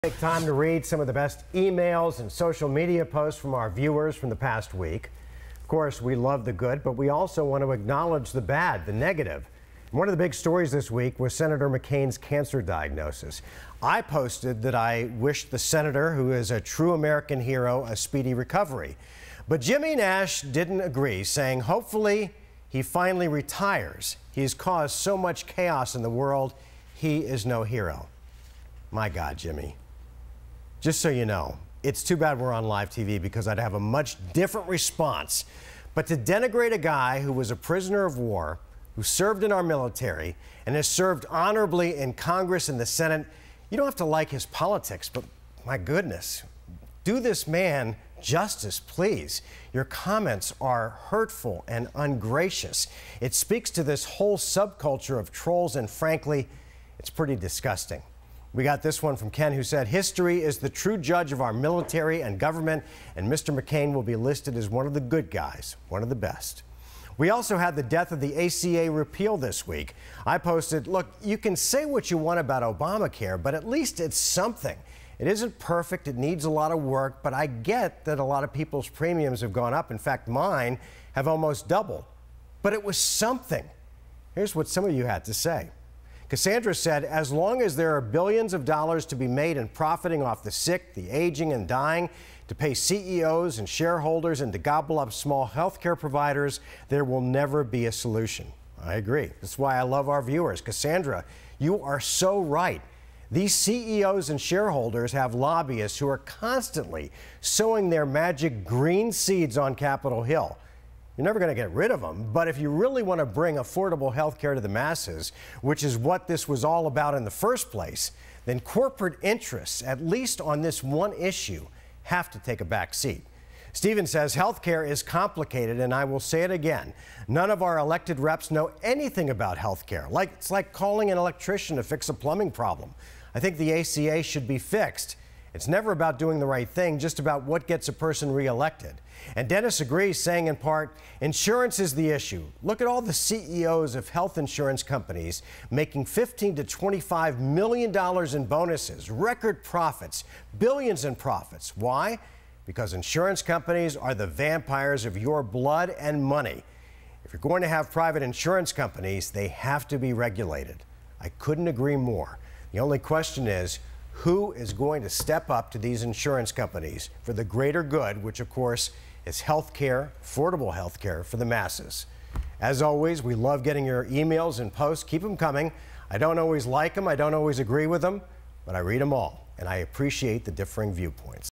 Mark Curtis responds to viewer comments
Mark Curtis responds to viewers social media comments and email pertaining to biggest news stories of the week.